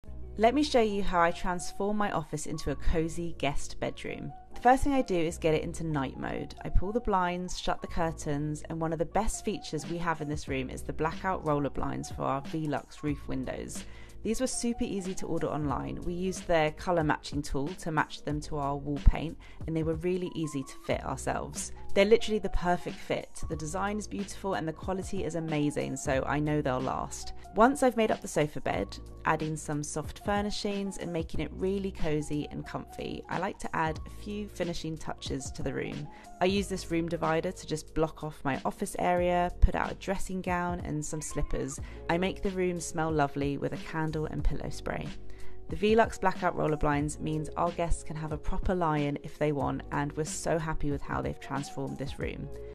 Guest Bedroom Transformation with Lily Pebbles
Watch how Lily transforms her home office into a cozy guest bedroom